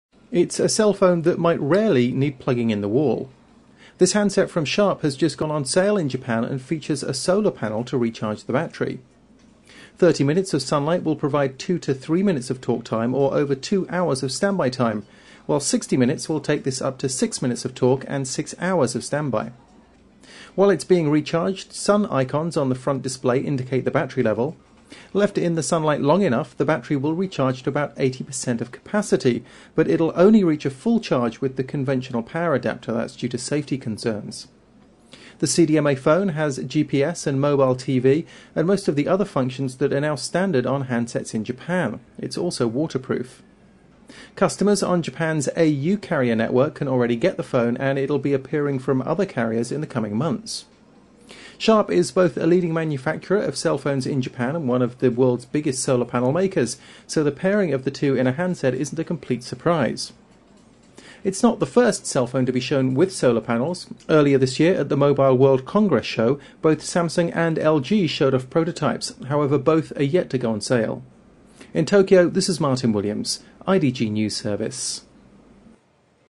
Sharp's latest cell phone recharges with a solar panel
An hour in the sun will provide 6 minutes of talk time and 6 hours of standby time. The phone has just gone on sale in Japan.